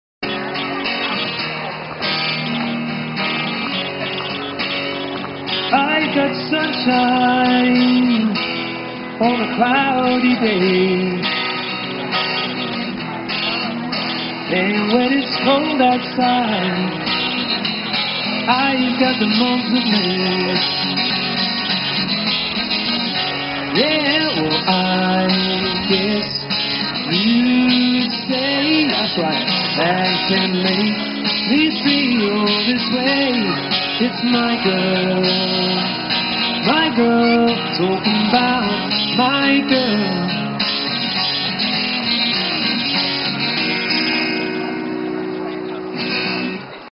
Street Artist
Street Artist in Liverpool City Centre